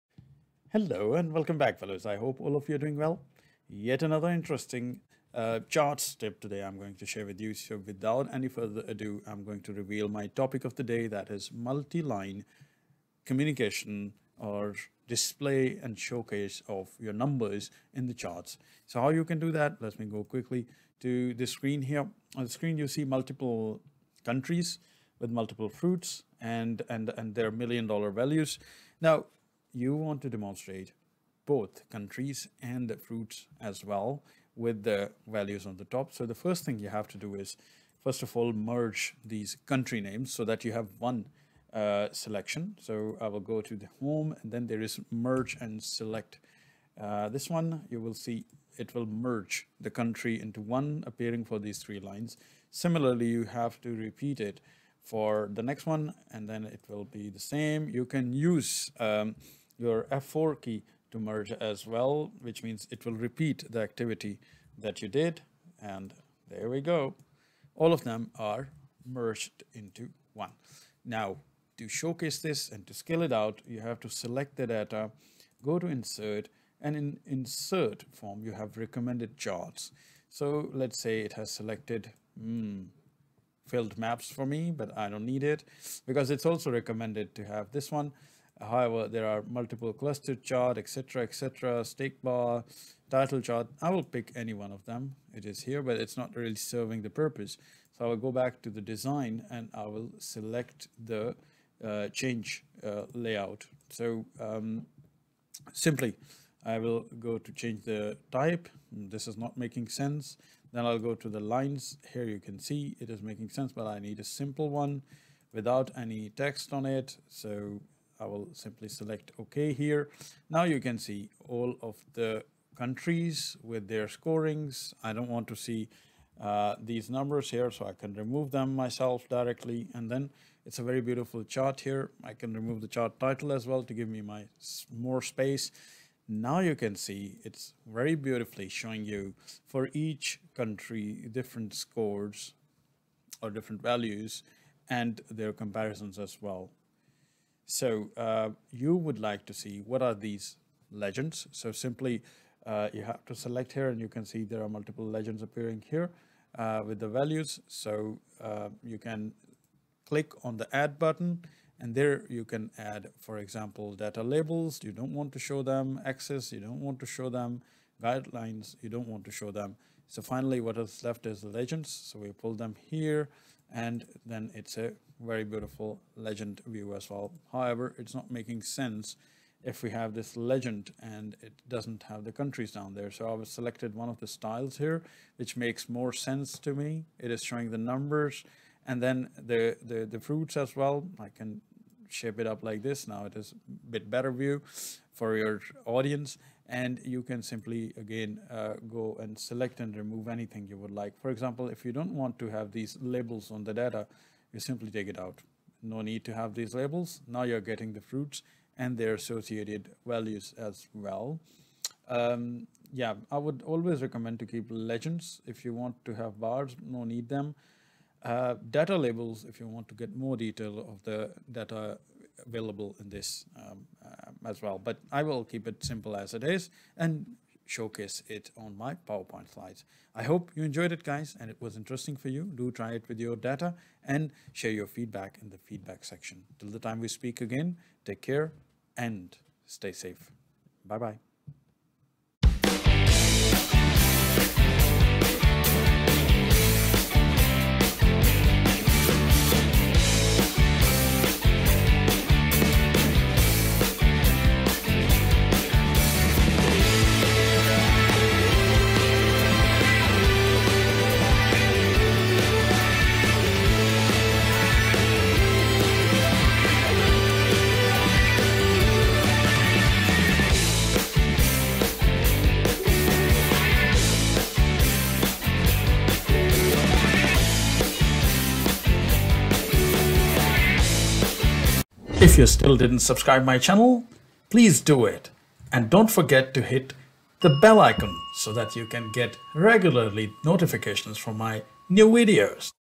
How to Create a MULTI-LEVEL CATEGORY chart in Excel?
Quick tip on how to Create a MULTI-LEVEL CATEGORY chart in Excel?